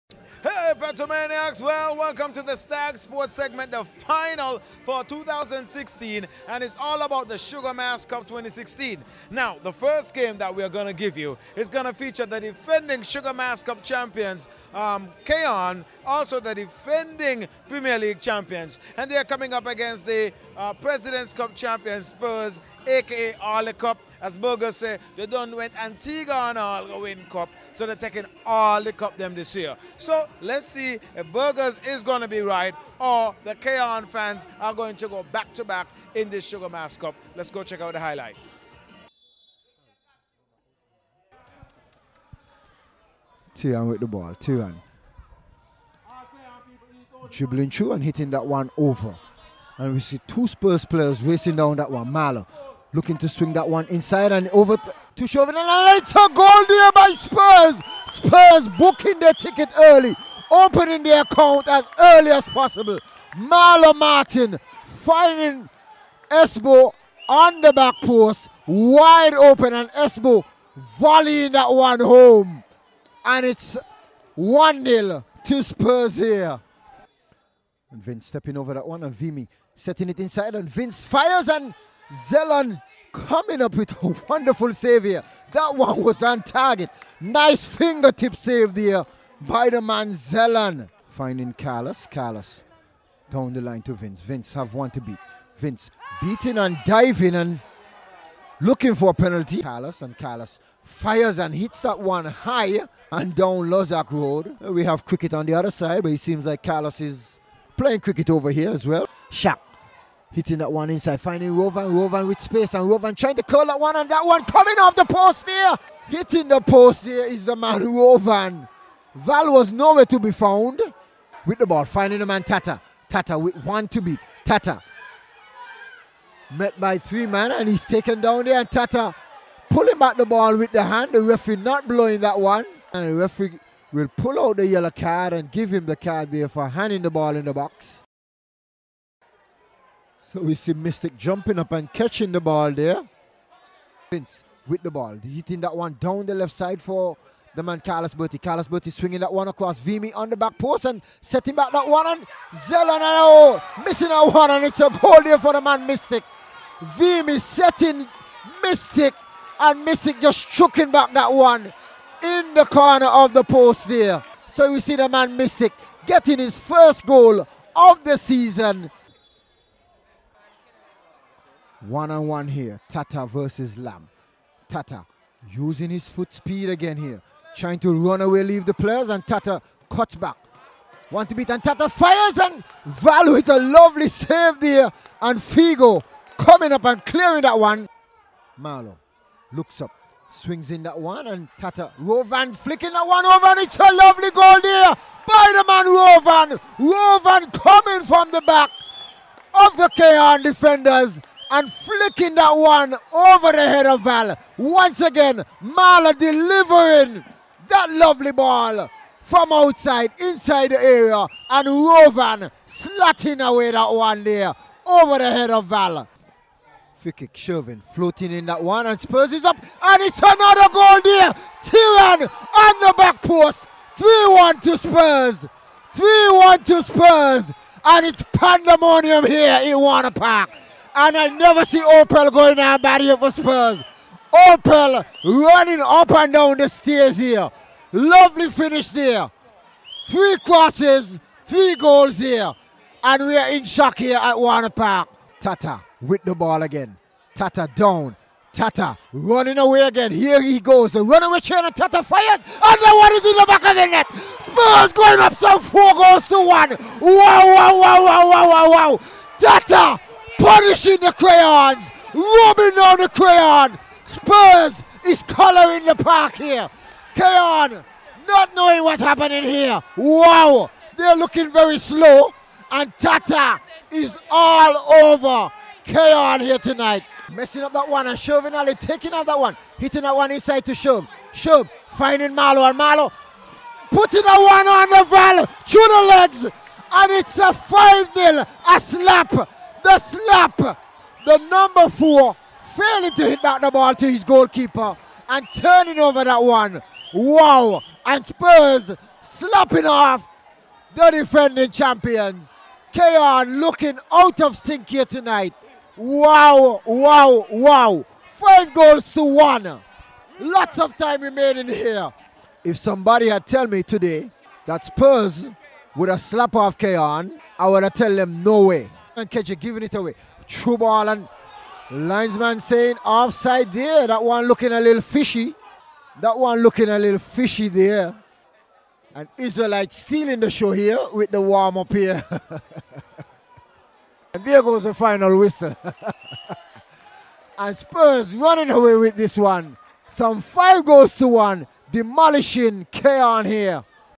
Spurs slap Cayon
Spurs set the 4-1 beatin they got from Cayon and Bruk it with a 5-1 Slappin